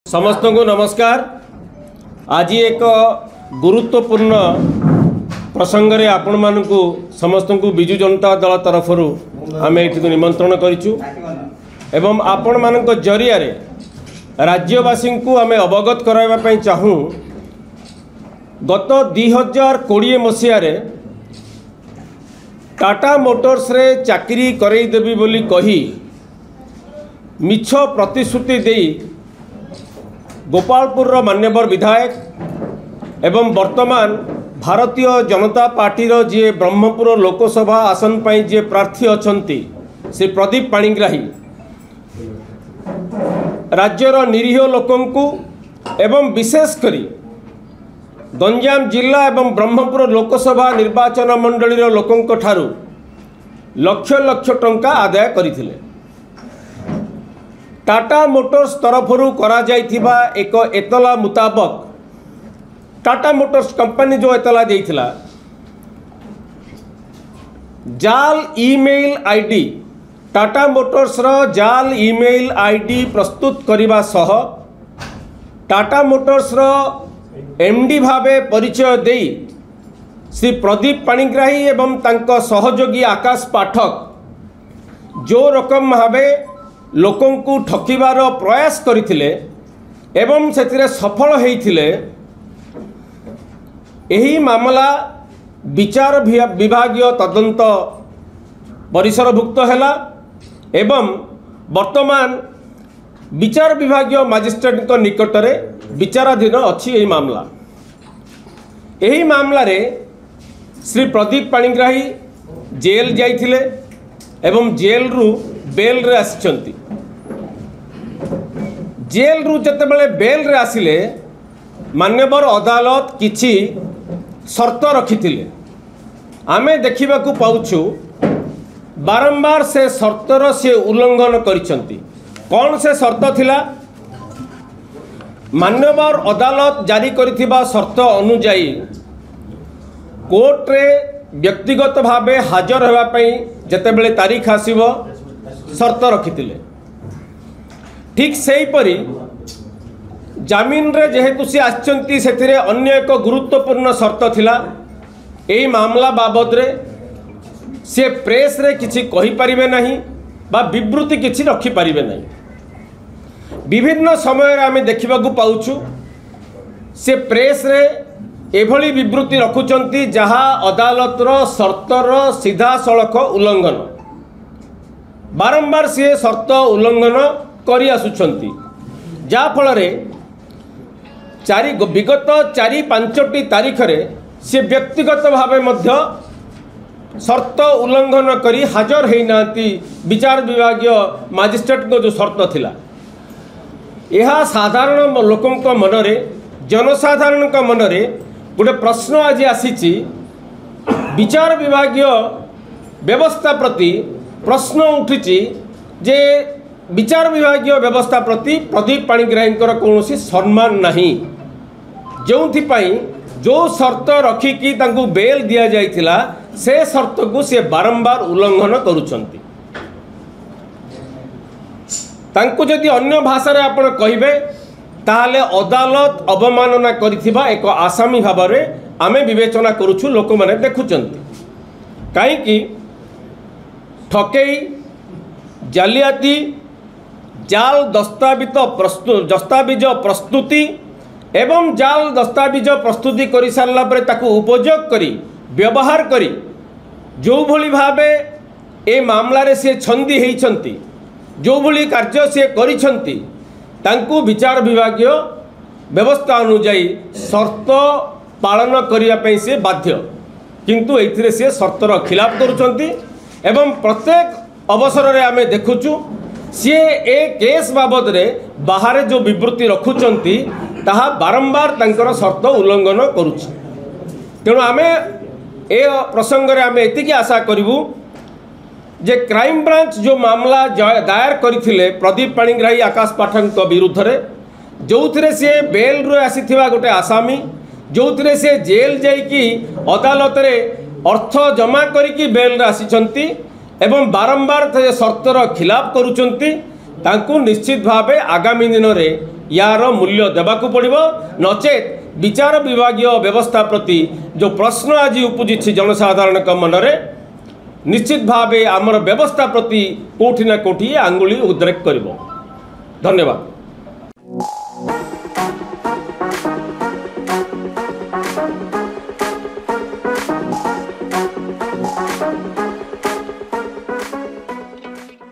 Pradeep Panigrahi Defying Orissa High Court Order | Watch BJD Spokesperson Tumbanath Panda Reaction
Watch BJD Spokesperson Tumbanath Panda Reaction.